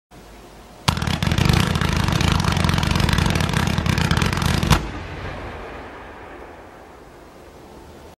Επαγγελματικό Πυροτέχνημα Βεντάλια Golden Blue Green Three Layer Mine 100 Βολές
Επαγγελματικό Πυροτέχνημα Βεντάλια Golden Blue Green Three Layer Mine
Αθόρυβο Πυροτέχνημα
Πακέτο Πυροτεχνημάτων 100 Βολών
Διαμέτρημα 1.20''
Κατηγορία F4
Κωδικός: YT-4104-C